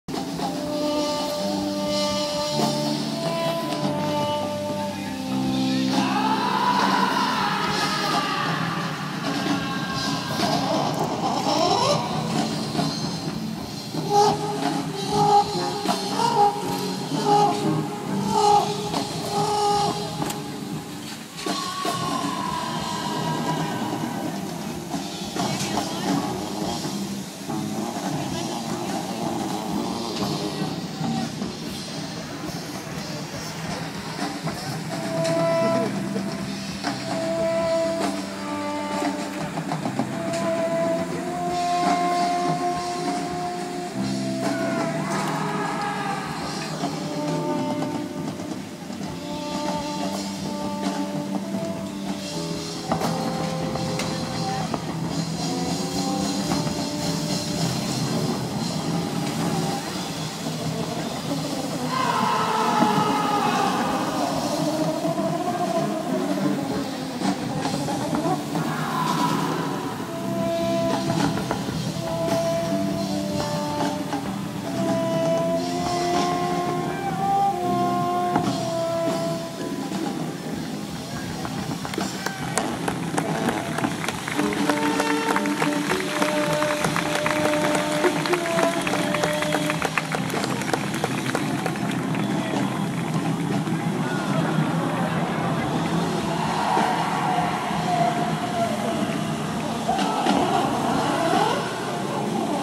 Santiago a Mil: Titanic: Part 6
Part of the 2012 Santiago a Mil festival. The Titanic sinks and breaks apart.